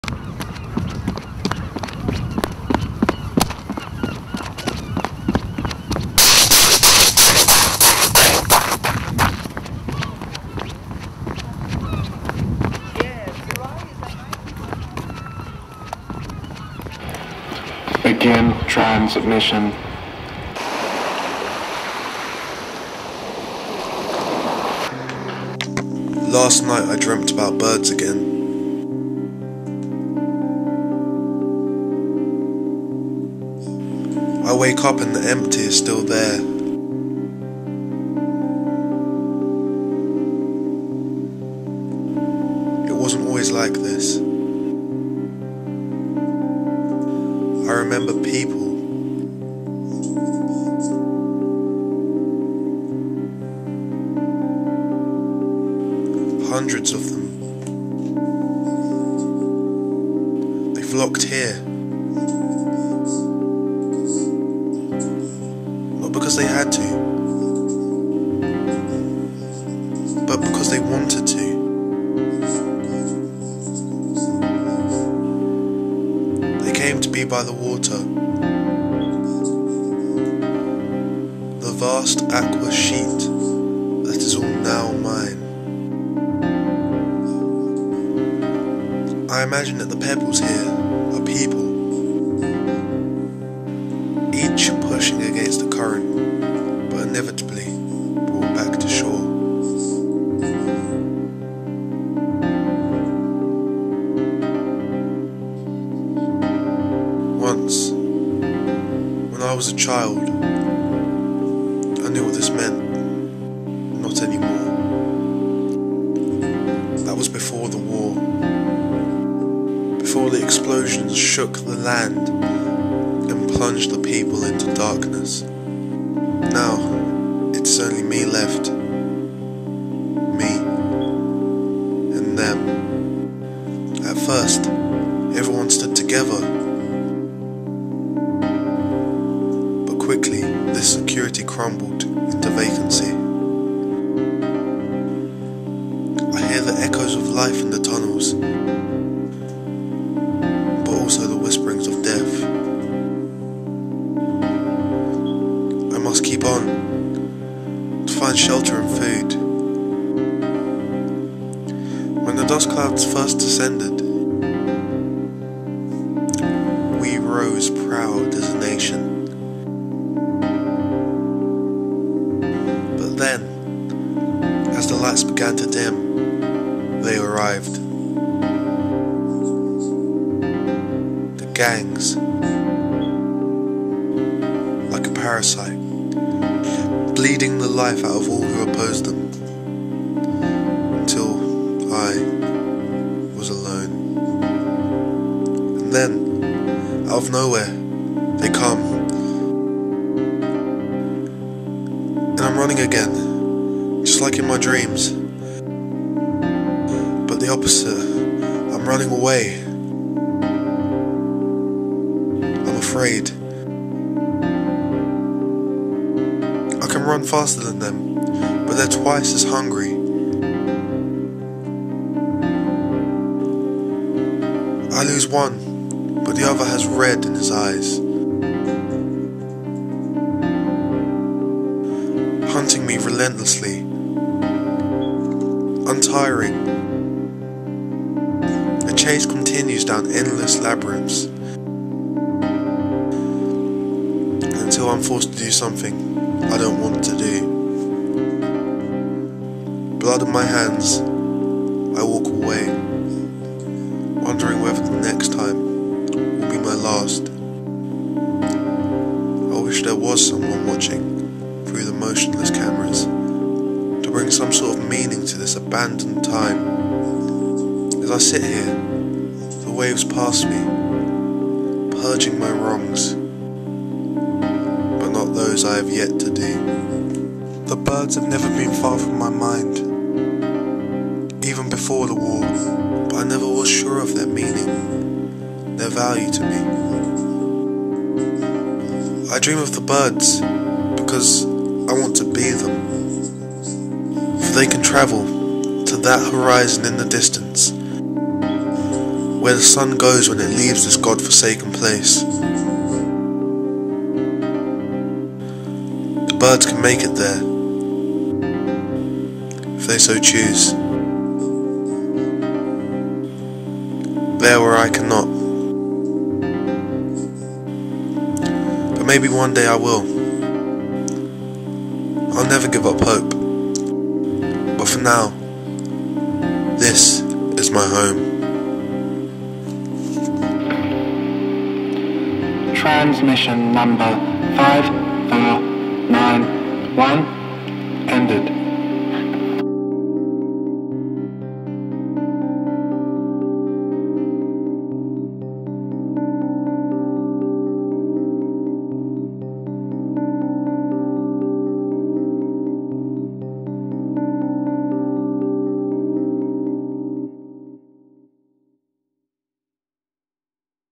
Home
A young man muses on his existence and his surroundings in his 5491st transmission since the War changed his Home forever.

Youth Animation and Media 2010

Daniel Rands
Billy Gordon-Orr
Jacob Stuttard
James Lalor
Ella McCull
Ellie Ivens

Tutored by Alison McCloskey